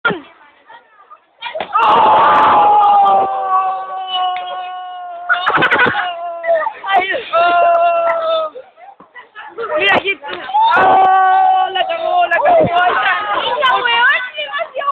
la caida de najle
nagle con esto te volveras famoso es la mejor caiiada del año jajajja es ta pa cagarse d la risa o eso creo